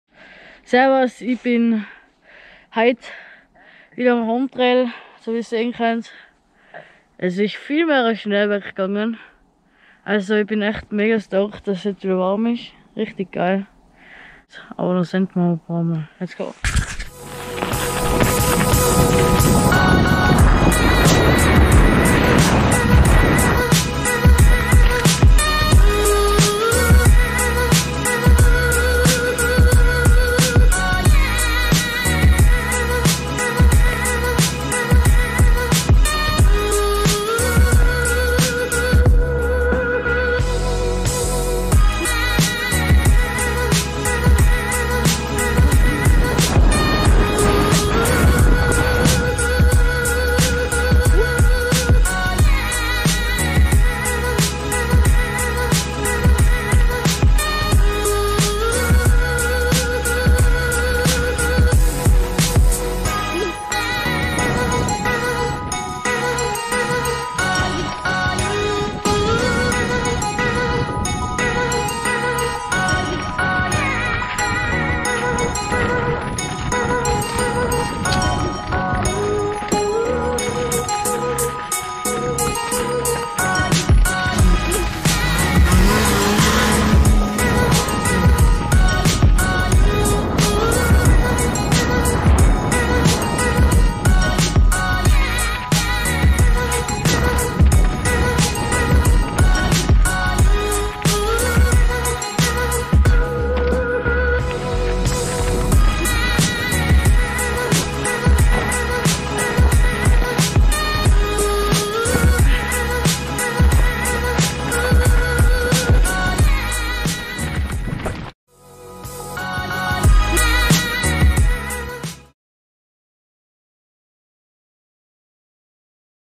snowriding❄ and fall riding🍂
the snow hasn't  gone  compleatly but there was Not too much snow so i had lots of Fun in the Woods